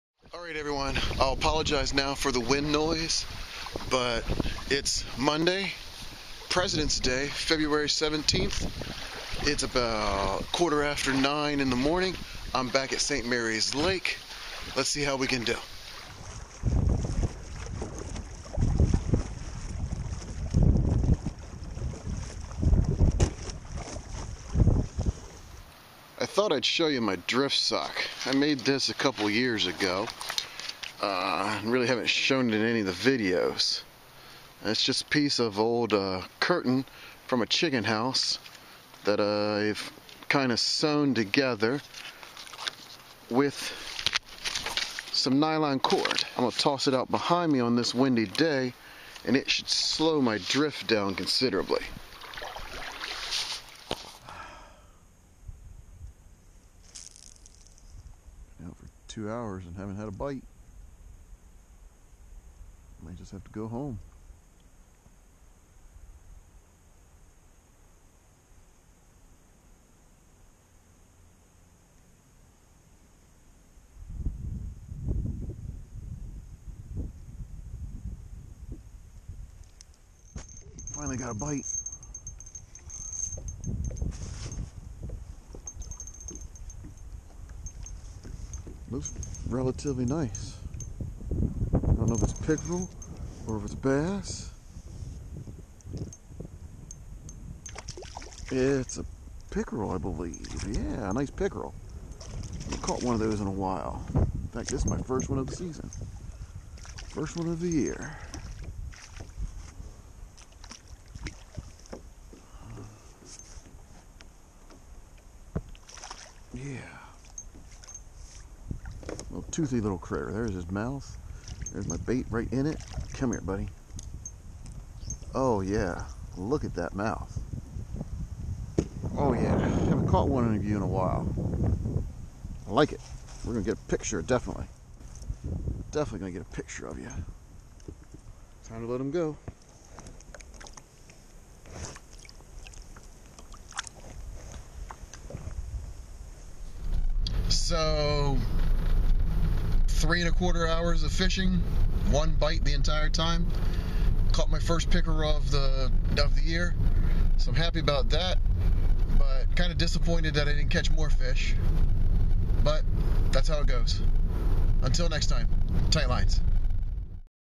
FSMD: Fishing Shorts Season 3 Episode 2
With the President's Day weekend going by so fast I really only got to try fishing Monday morning.  Unfortunately the bites were very very few and far between.  In fact I only got one bite the entire time I was out on St. Mary's Lake.  I was able to land the fish, my first chain pickerel of 2020.  Hope you enjoy this "Fishing Shorts!"